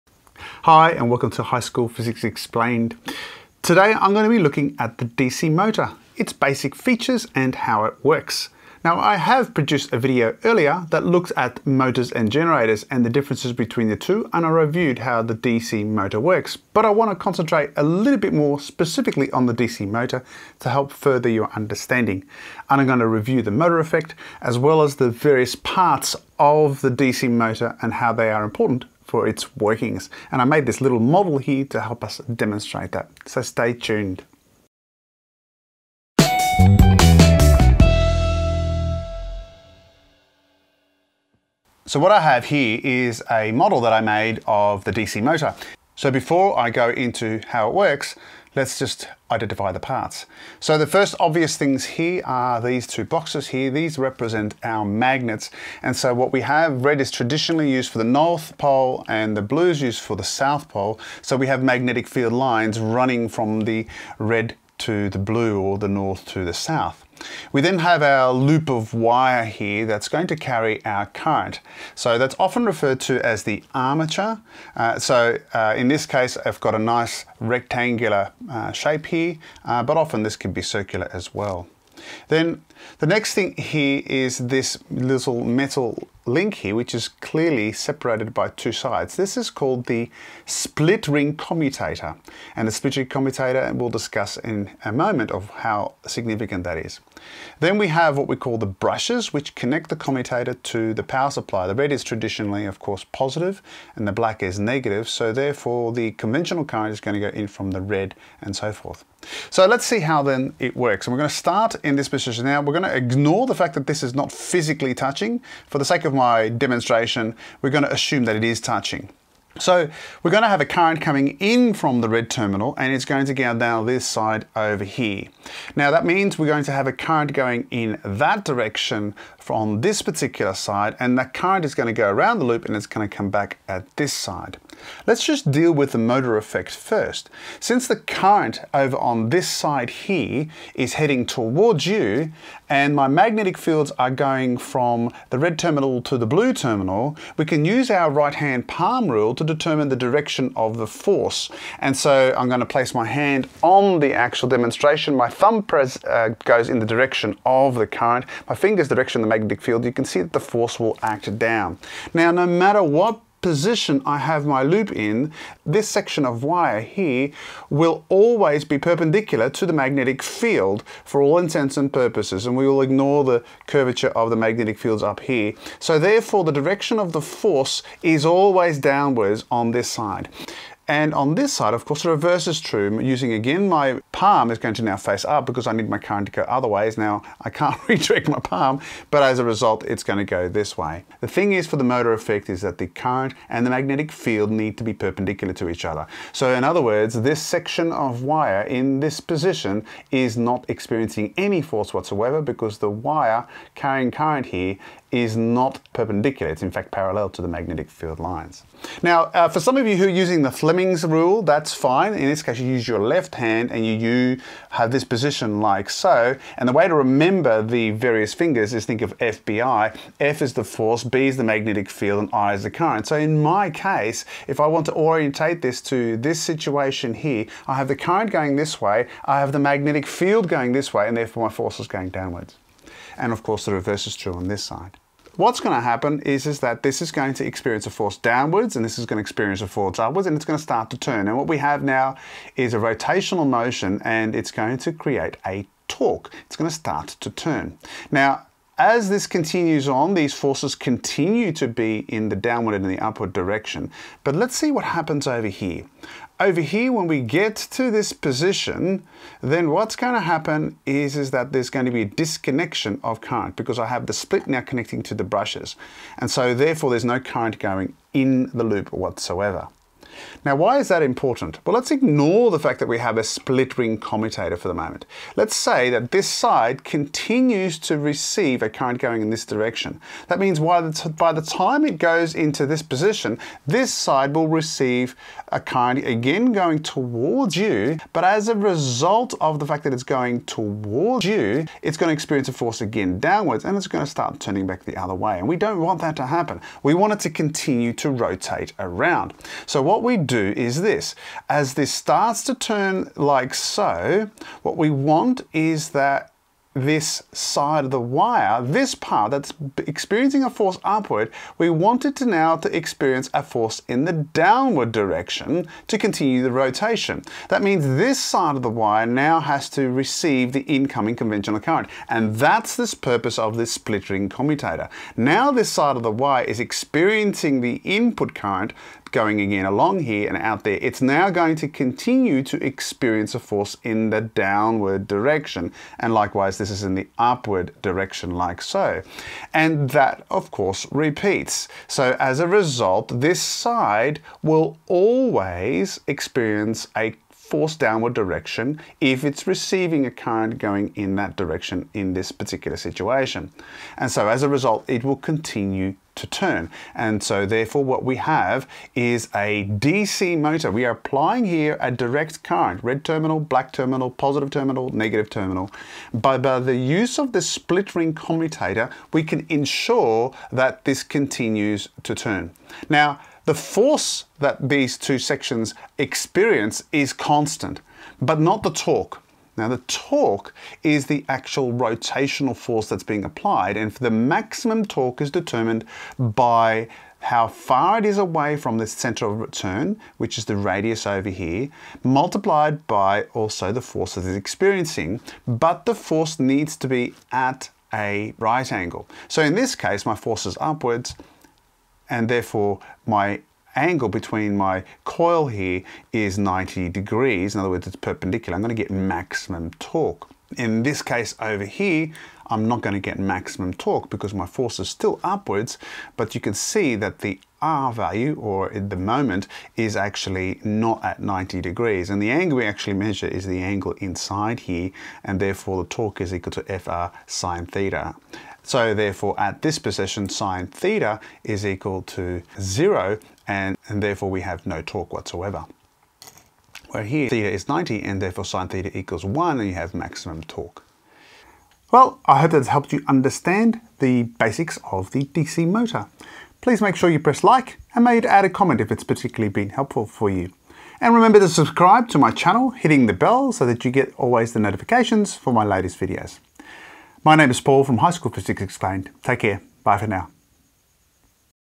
How do DC motors work?
Using a model , I explain the basics of a DC motor; how it relies on the motor effect and the importance of the split ring commutator for its working.

And please add a COMMENT to let me know I have helped you.